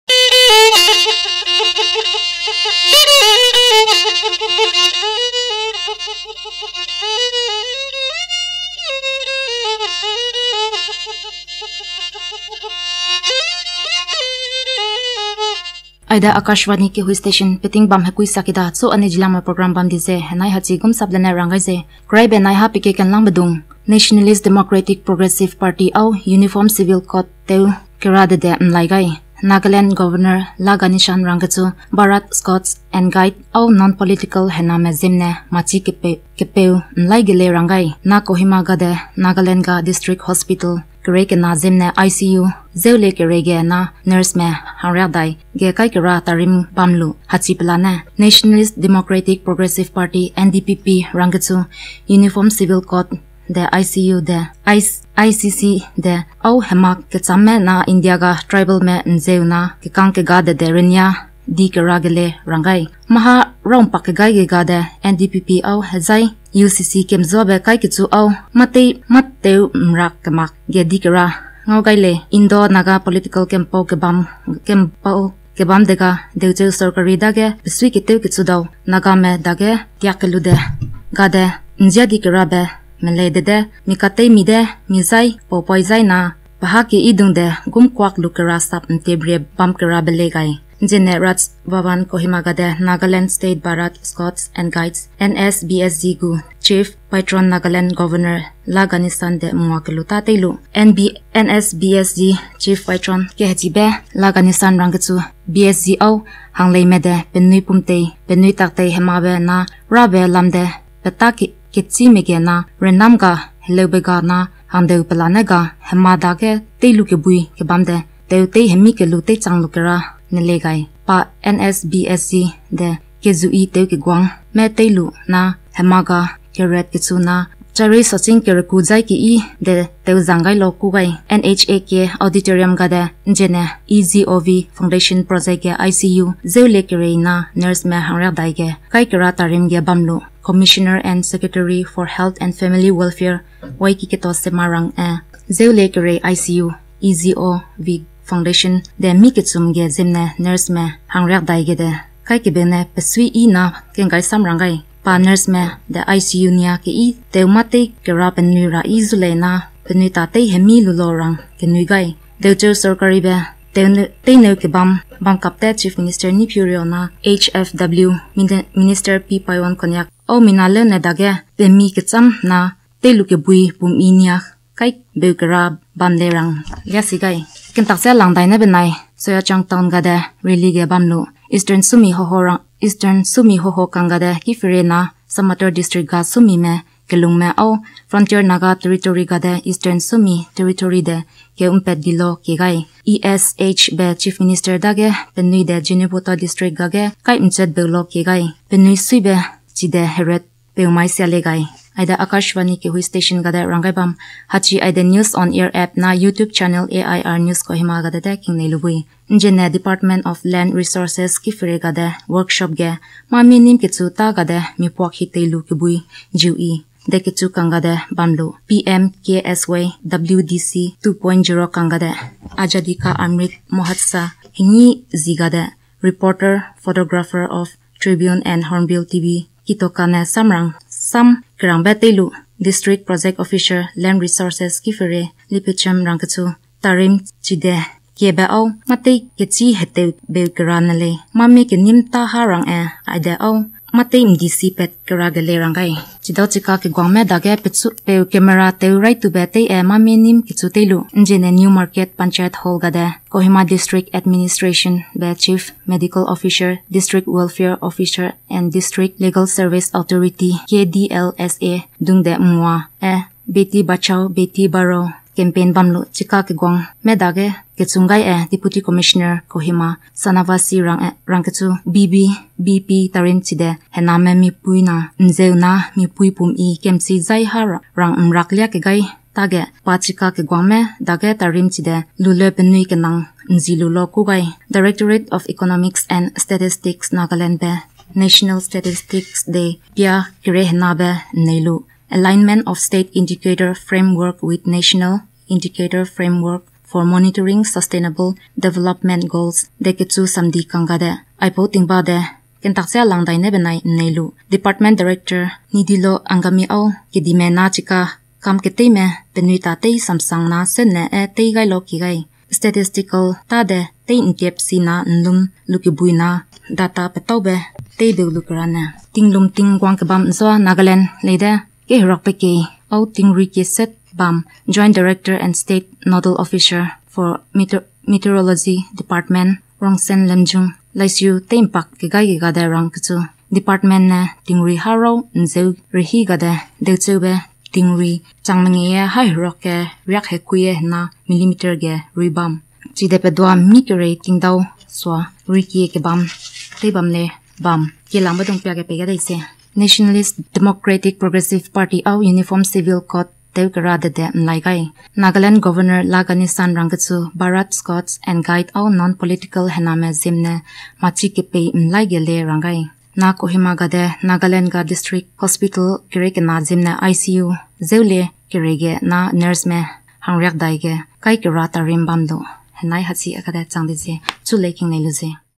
Akashvani News Kohima Zeliang Bulletin on July 1, 2023.
:: THE HEADLINES ::

 1.   Nationalist Democratic Progressive Party voices against implementation of Uniform Civil Code.

2.   Nagaland Governor La Ganesan says Bharat Scouts & Guide is a voluntary non-political, educational movement for young people.

3.   And, Induction programme of 40 nurses of 10BedICU  for 12 district hospitals of Nagaland held in Kohima.